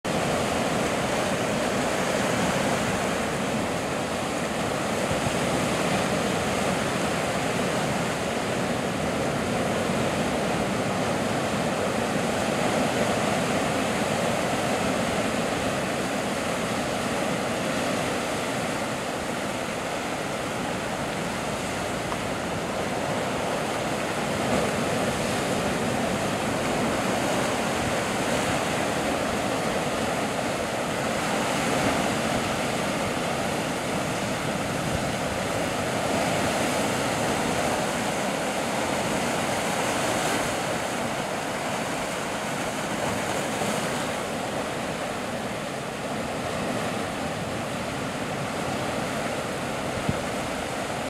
Calm, blue water (different inlet)
A second cove along the Pacific coastline, western Oahu. No color enhancements or other editing. What you see is how it is. Video taken just before low tide, when the water is its calmest.